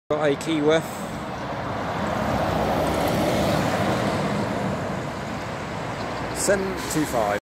Trent Barton red spare Volvo B7RLE Wright-Eclipse Urban 725 on The Keyworth
Red spare Mango Volvo B7RLE Wright-Eclipse Urban seen heading on Melton Road with a service for Nottingham (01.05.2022)